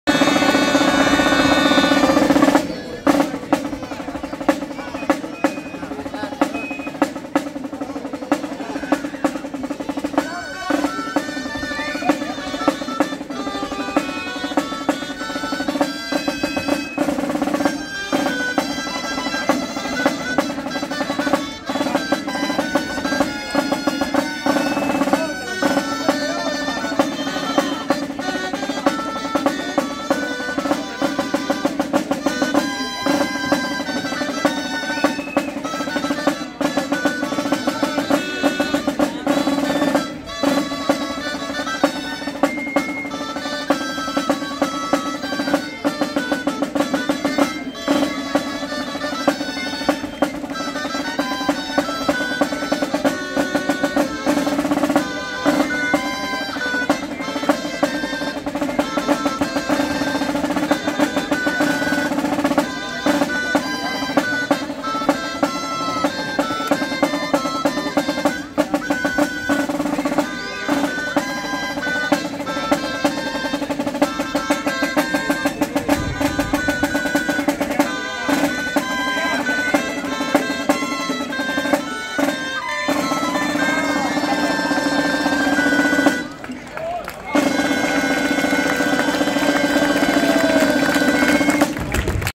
GIGANTES BARAÑAIN
LA COMPARSA DE BARAÑAIN , BAILA EN EL DÍA DE LA COMPARSA